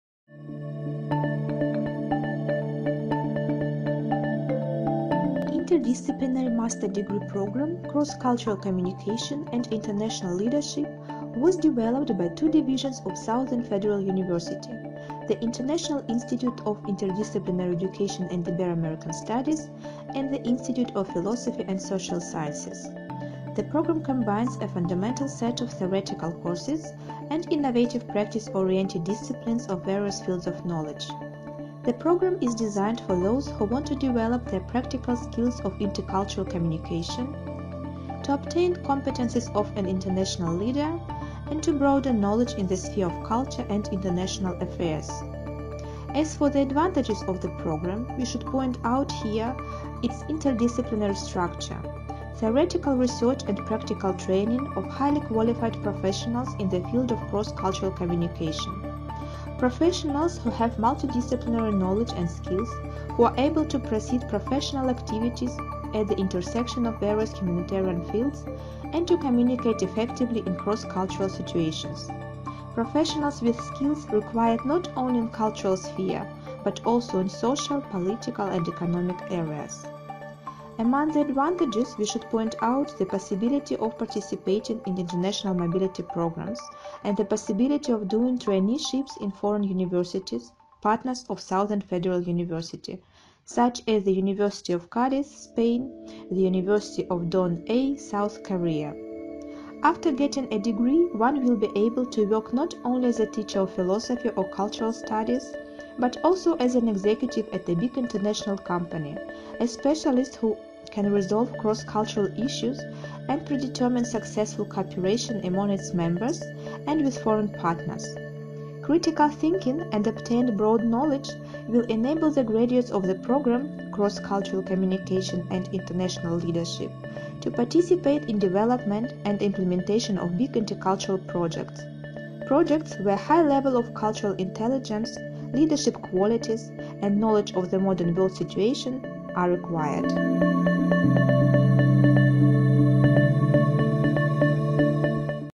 Master degree program Cross cultural communication and international leadership (English)
Degree: Master

Program duration: 2 years

Type of attendance: Full-time

Language: Russian, English / Spanish

Credits: 120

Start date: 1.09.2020

Location: Rostov-on-Don, Russia

Admission requirements: Bachelor’s Degree, Competence in English B1

Programme overview

Interdisciplinary master’s programme «Cross-cultural Communication and International Leadership” is aimed at theoretical, research and practical training of highly-qualified professionals in the field of cross-cultural communication, who have multidisciplinary knowledge and skills, who are able to proceed professional activities at the intersection of various humanitarian fields and to communicate effectively in cross-cultural situation. The program studies the problems of forming an international leadership during contemporary cross-cultural dialogue through the dynamics of its development, based on  theoretical, empirical and territorial approaches, which allows to predict possible development, to develop practical skills of intercultural communication, which are used not only in culture but in the social, political and economic processes of the modern world.

Research areas:

Language and cross-culture. Formation of the language personality of the international leadership. Speech strategies and tactics of a leader: persuasion and manipulative technologies in modern culture. Cross-cultural competencies of an international leadership.

Programme structure

The program combines a fundamental set of theoretical courses and a list of practice-oriented disciplines of various fields of knowledge and contributes to the formation of methodological tools which are necessary for the effective implementation of work in various social spheres (culture, education etc.).

Typical units of study may include:

·Philology in the system of humanitarian knowledge.
·Foreign language / Theory and history of culture.
·The modern models of culture research.
·Problems of cross-cultural communication.
·Oratory and culture of speech.
·Paradigm of modern humanitarian knowledge: cultural studies, socio- and psycholinguistics, pragmatics.
·Social and cultural anthropology.
·Cultural branding of territories.
·Cultural policy and culture of interethnic relations.
·Сultural linguistics. Image study.
·Human identity: social and philosophical aspects.
·Personal identity and gender theory.
·International legal communication and language policy.
·Second foreign language (Spanish / English).
·Business foreign language (Spanish / English).
Employment

¨ Scientific and teaching activities.

¨ Expert and advisory activities on language, cross-cultural, international policy and inter-ethnic and intercultural relations.

Careers:

¨ Diplomatic missions and consular offices.

¨ International companies,

¨ Enterprises, which develop intercultural relationship.

¨ Russian and foreign universities and schools, various cultural and educational centers.

¨ Advertising agencies.